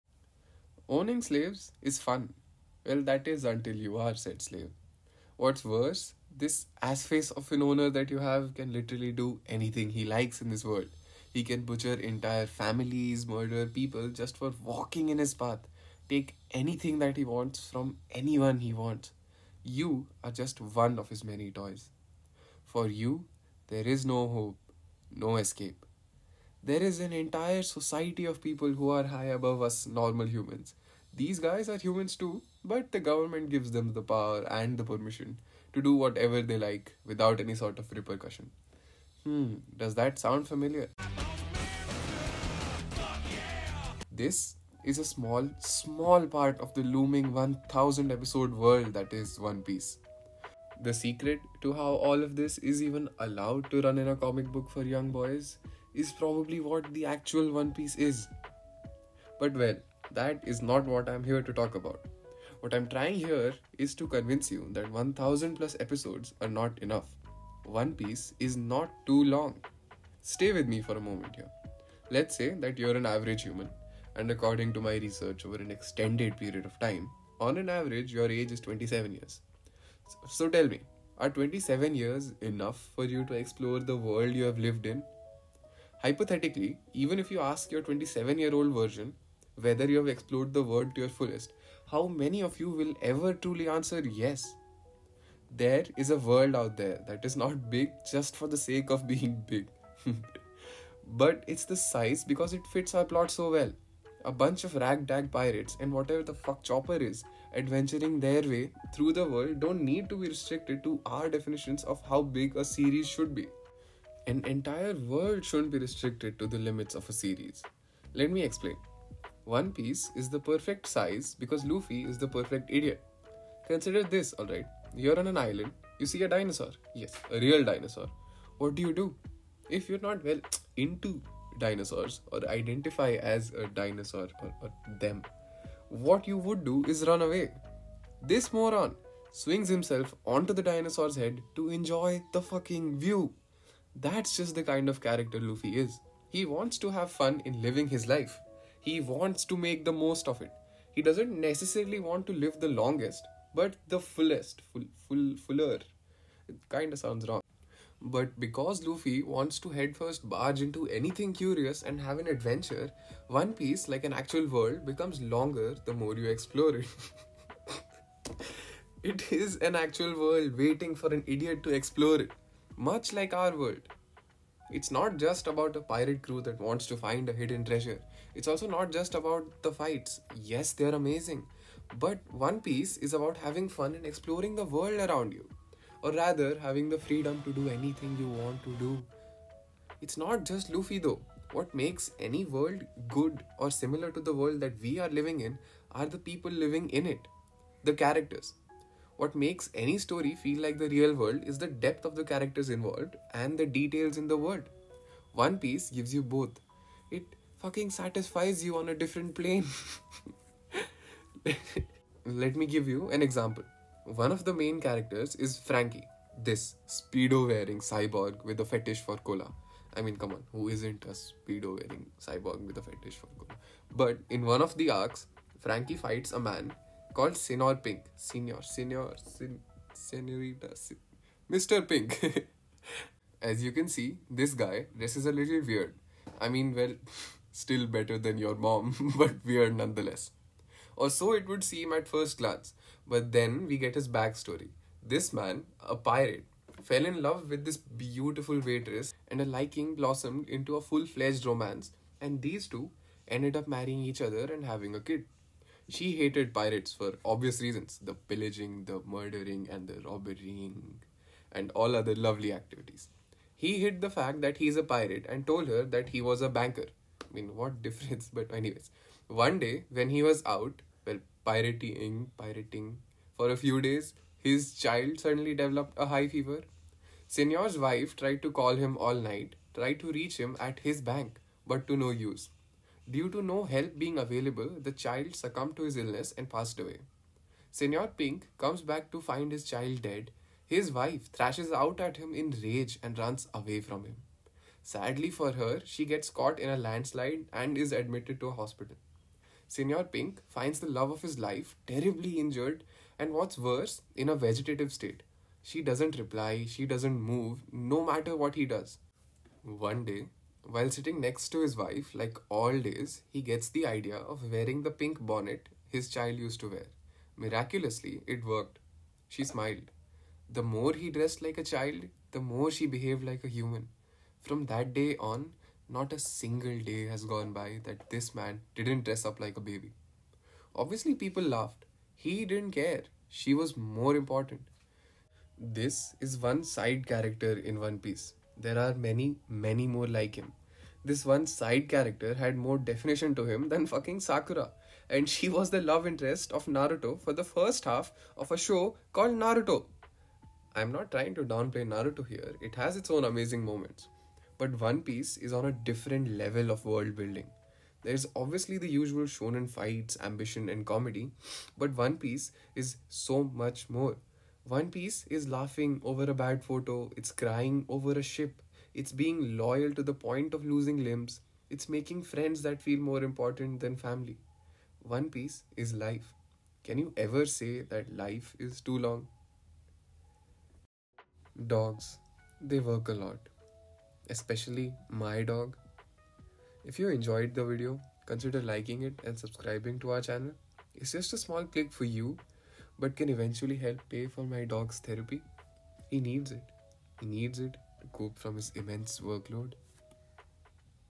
One Piece is too short.
The first thing that comes to mind when we think of one piece is its length. ;)
I’m about to show you guys that it’s actually not how long you think it is.
It’s infact too short.